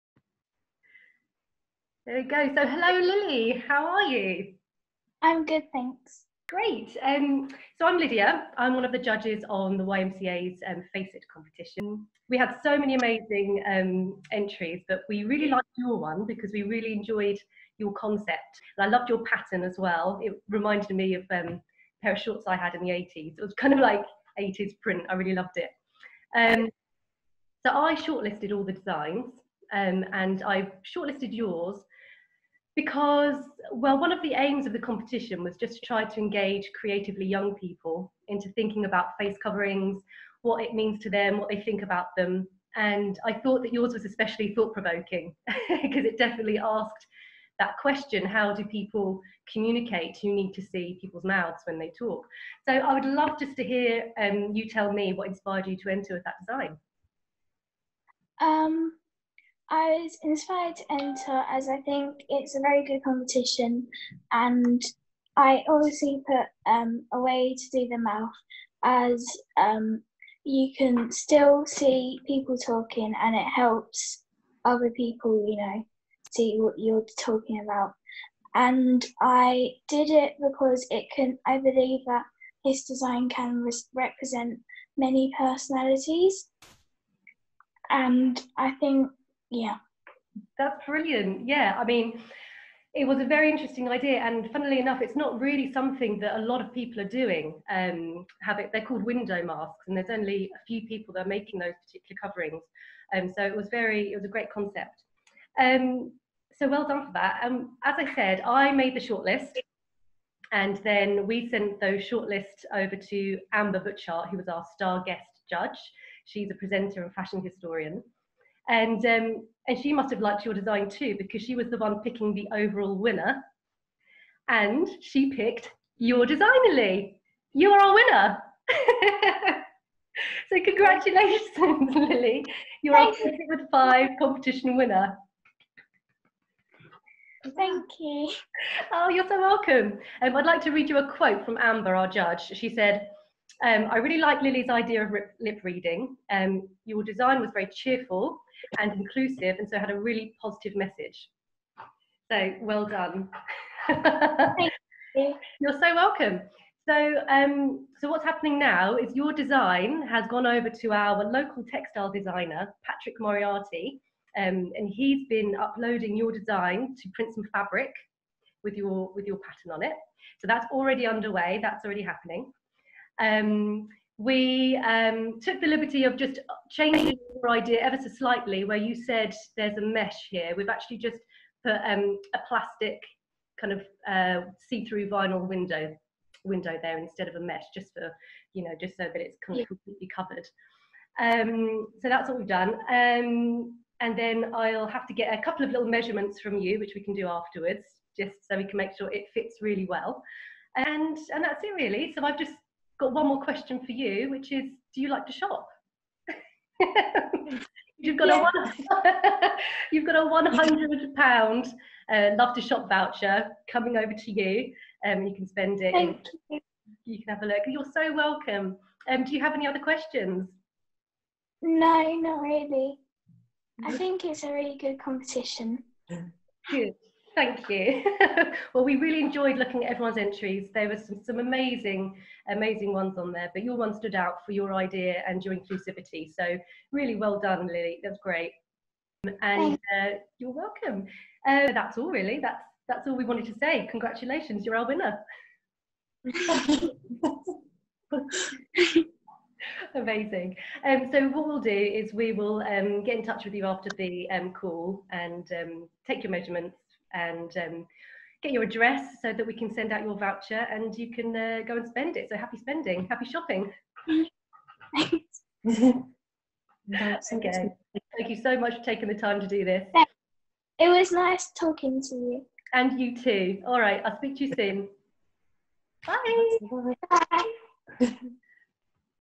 Face It Competition Winner
Here's the moment we told Lily that she is the winner of our Face It Competition! 🎨✏️Congratulations Lily, we were so impressed by your design! Lily said “I think it was a very good competition!